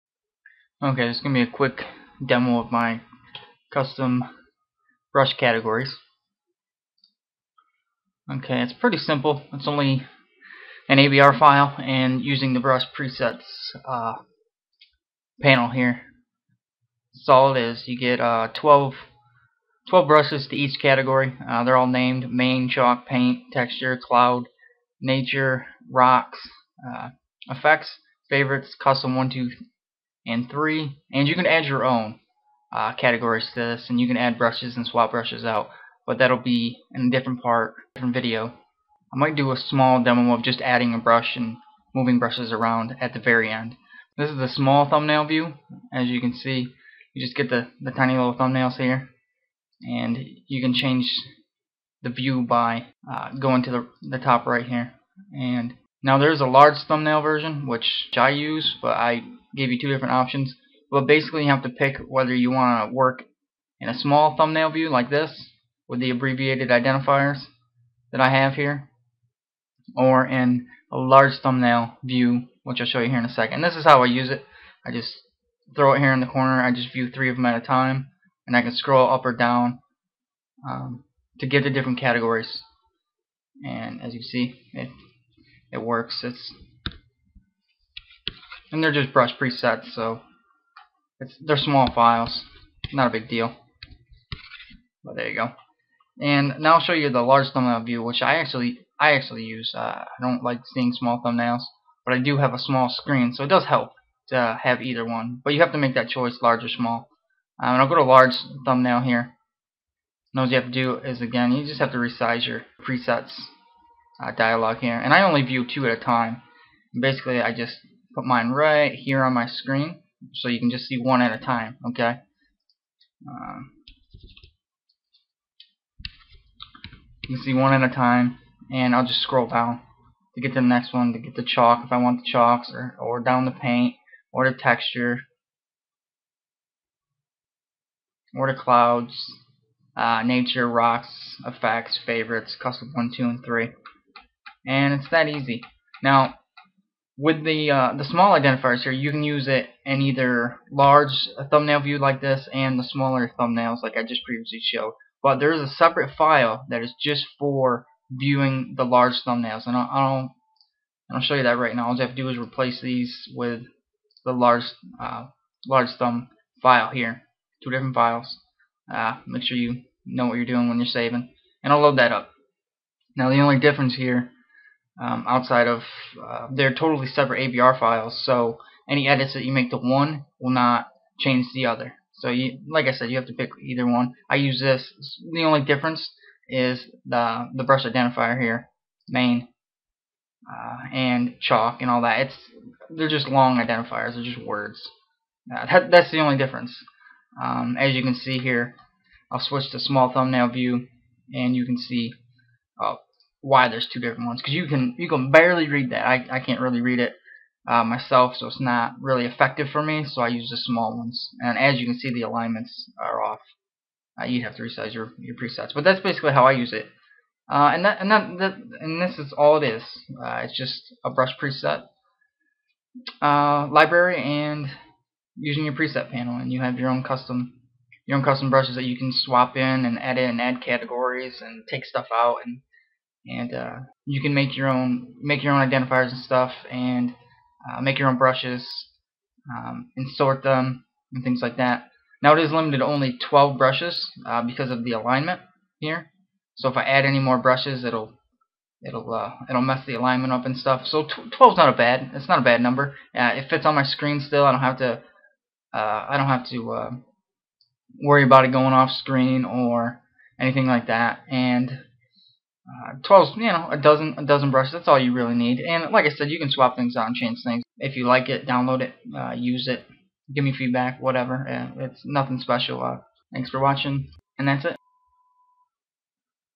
FREE! drbjr Custom Brush Categories - Photoshop Brushes
! DOWNLOAD top right THAT page!
Simple and easy way to sort your Photoshop brushes
2 .abr brush files you can choose between a small thumbnail or large thumbnail set up - See demo or deviantart link for a visual
Comes with 12 different categories Main,Chalk,Paint,Texture,Clouds,Nature,Rocks,Effects,Favorites,Custom 1/2/3
12 Brushes to each category based on the use.
You can Add/Remove/Swap/Rearrange brushes and categories. There is a full video tutorial Included: I walk you through customizing your brushes and categories.
There are also some backup files just in case.
FOR FREE: But you can donate a little something if you want. Thanks
COMPATIBILITY Please READ:
Photoshop Only - No I will not convert or make them for any other software
They are just .ABR files should work just fine on windows/mac:)
As far as versions of Photoshop, I use CS6/CC but it should work on earlier/later versions
!Always save your documents at regular Intervals - I am not liable for anything lost/broken/corrupt Or anything else.!
Installation Instructions:
Super easy
1. Extract "drbjr-CBC" to where you can find it - Open Photoshop
2. With Photoshop Open - Open your BRUSH PRESETS panel - Menu Bar - Window - Scroll down and you will find it.
3. Once that panel Is open - Click TOP RIGHT corner - Scroll down to REPLACE BRUSHES - Search for "drbjr-CBC" folder you extracted.
4. Open "drbjr-CBC" folder - Choose from the 2 .abr brush files there - Large for full name Identifiers - Small for Abbreviated Identifiers - Click LOAD.
5. Once the brushes load - Change brush presets panel to small or large thumbnail view - Click top right corner and scroll down - choose "Small Thumbnail" or "Large Thumbnail" -Resize the brush presets panel until the category Identifiers line up on the left had side.
6. You are done - There is also a full video tutorial Included were I walk you through customizing/Editing the categories/Adding/Removing brushes and categories.